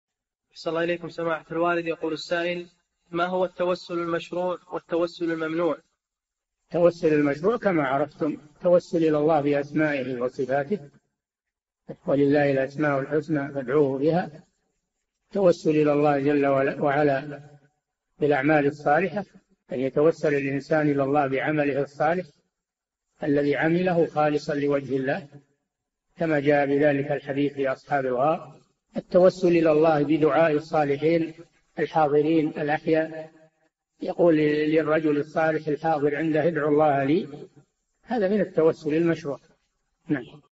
[1923- 4717] ما المراد بالتوسل المشروع والتوسل الممنوع؟ - الشيخ صالح الفوزان
[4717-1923] ما هو التوسل المشروع والتوسل الممنوع؟
الجَوَابُ: التوسل المشروع كما عرفتم التوسل إلى الله بأسمائه وصفاته، ﴿وَلِلَّهِ الْأَسْمَاءُ الْحُسْنَى فَادْعُوهُ بِهَا﴾ [الأعراف: 180] التوسل إلى الله جل وعلا بالأعمال الصالحة أن يتوسل الإنسان إلى الله بعمله الصالح الذي عمله خالصا لوجه الله كما جاء بذلك الحديث في أصحاب الغار، التوسل إلى الله بدعاء الصالحين الحاضرين الأحياء يقول للرجل الصالح الحاضر عنده: ادع الله لي! هذا من التوسل المشروع.
السيرة الذاتية :
للاشتراك في قناة الشيخ صالح الفوزان
لمشاهدة حلقات فتاوى على الهواء كاملة

للاستفسار والإقتراحات والدعم
رئيس الفريق
لمشروع كبار العلماء